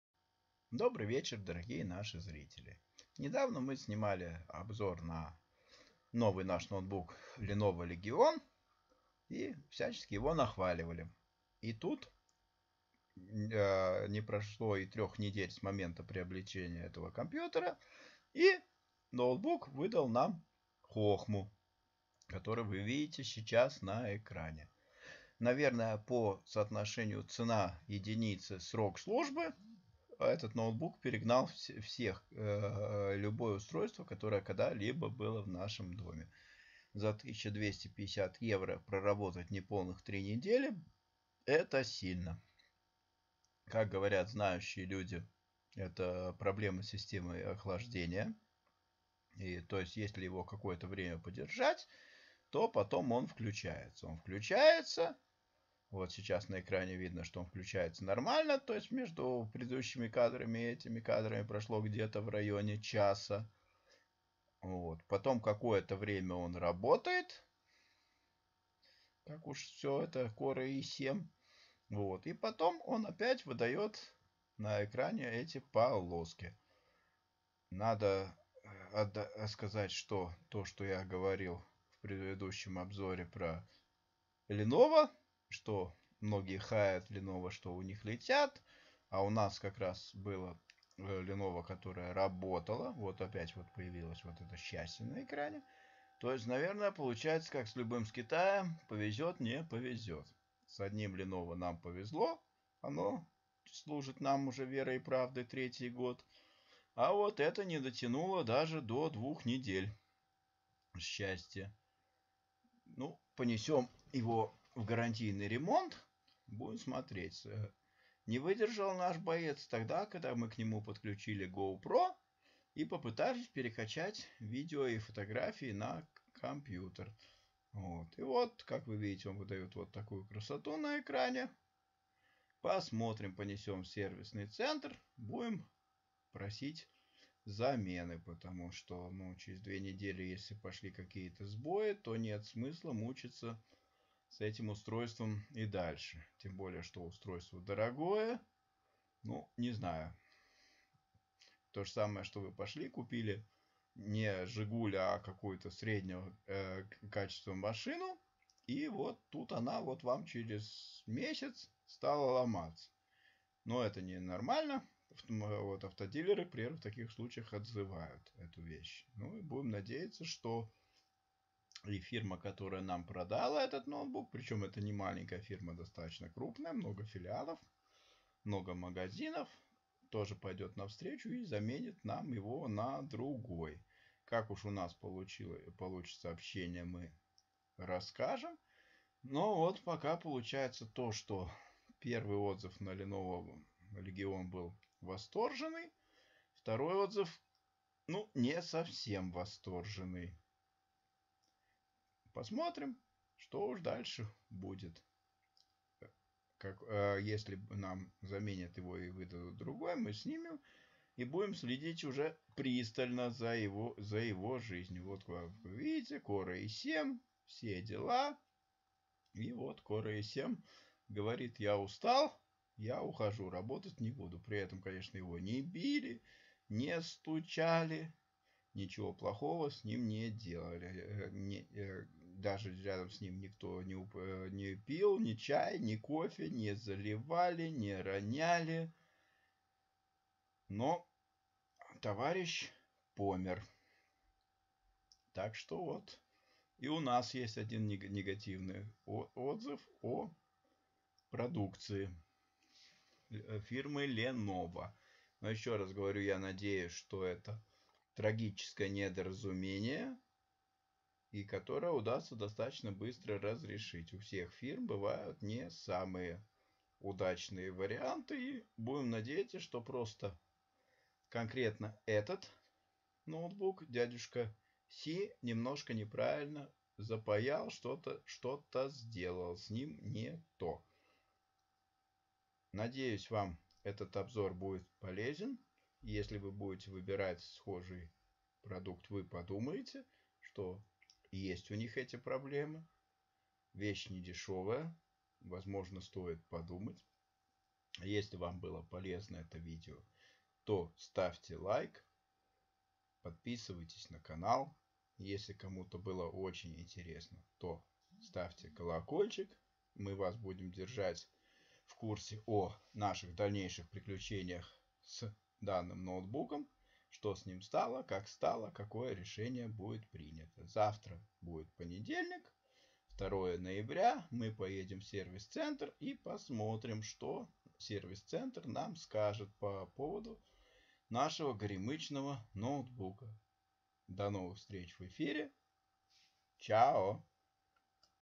Проработав неполных три недели Lenovo Core I7 Legion поломался.
Купили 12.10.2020. новый мощный ноут Lenovo Core I7 Legion. Ноут проработал чуть менее 3 недель и теперь поедет в сервис. Наш восторженный отзыв о Леново похоже был немного преувеличен.